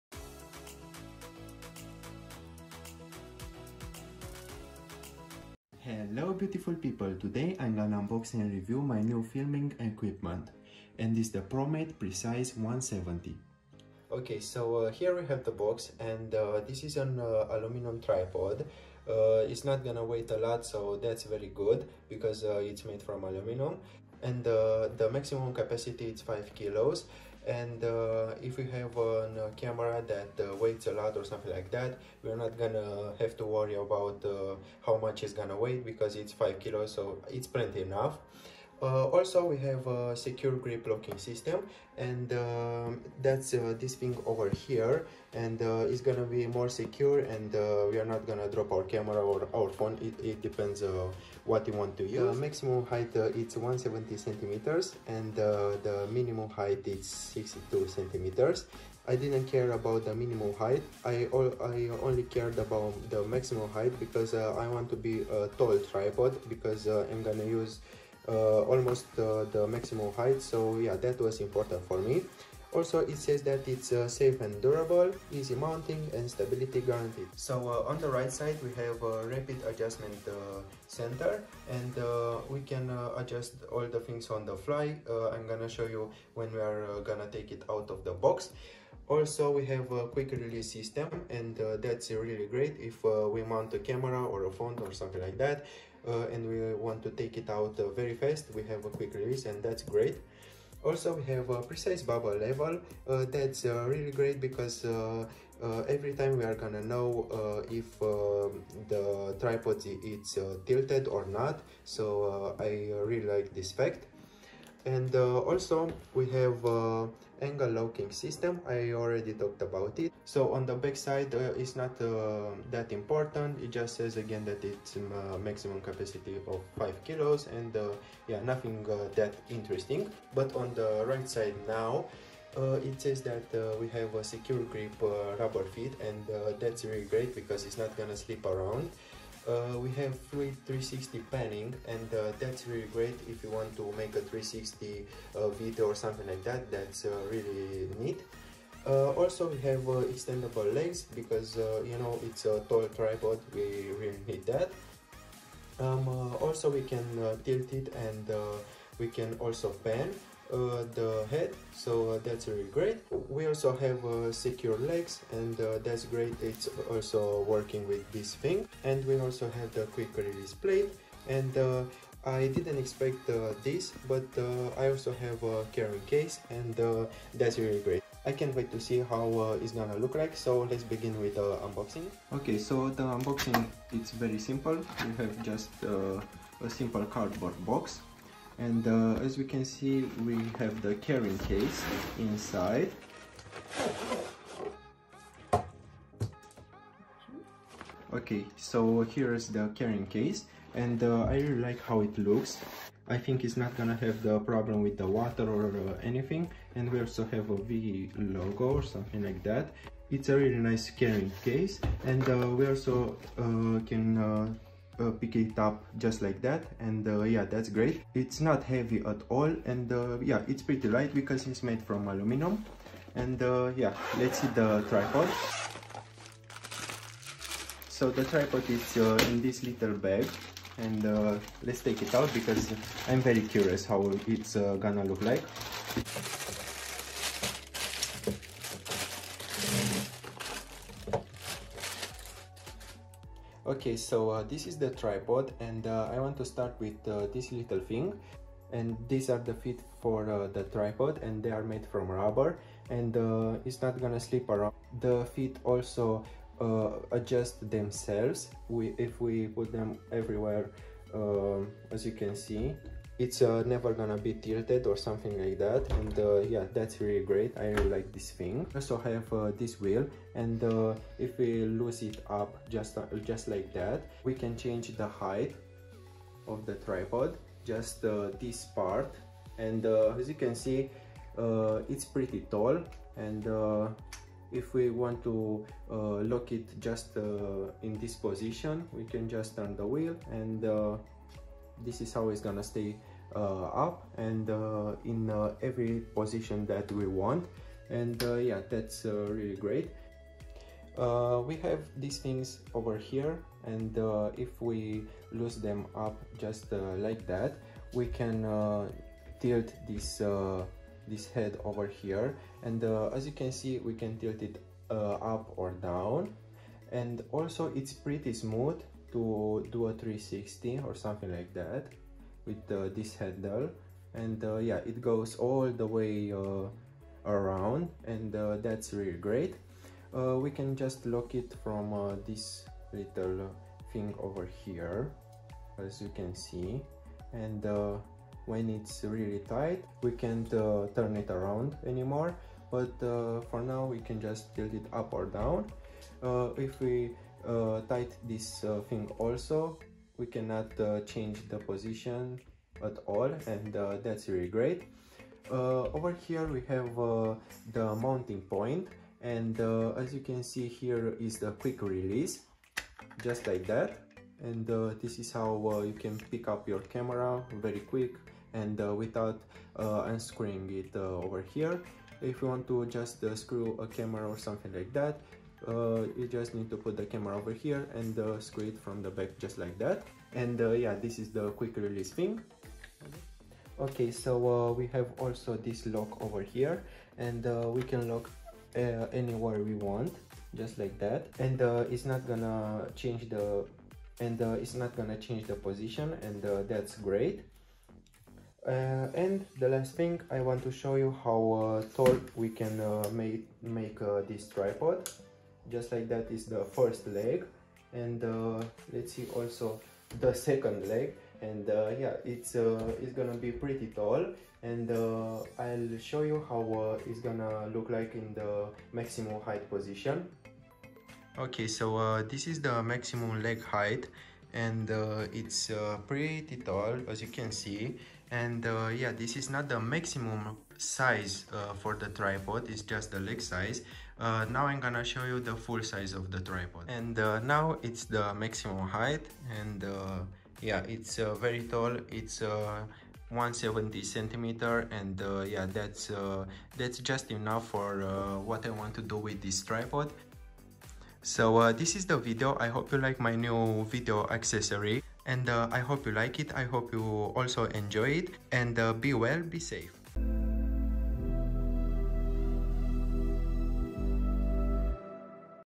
Best Budget Video Tripod? PROMATE PRECISE-170 UNBOXING & REVIEW
Today I am going to show you the PROMATE PRECISE-170! In this video I am going to do a quick unboxing and review, and I will also show you the carry case. In this video I will tell you how to adjust the tripod and if it's sturdy or not. In the conclusion of the video I can recommend this tripod without any problems and I think that if you'd like to record some videos this is a must have!